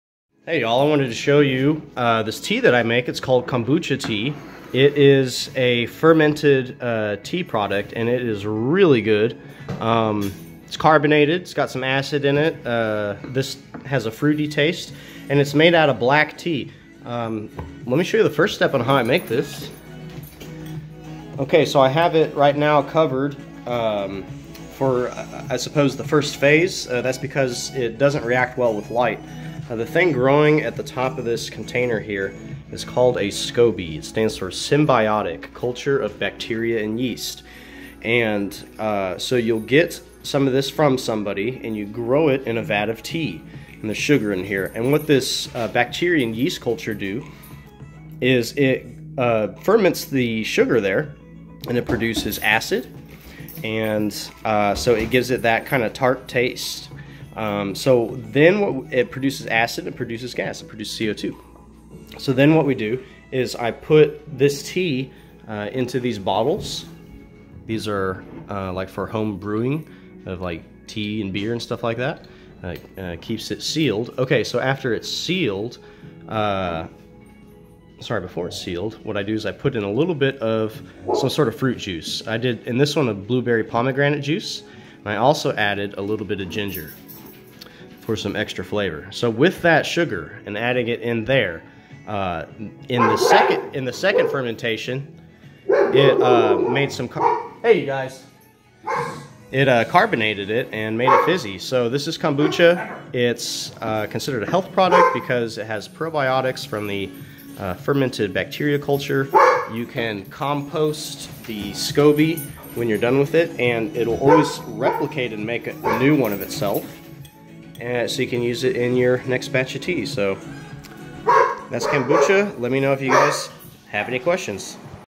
Kombucha tea overview
No demo. Just what I did and how I brew my kombucha.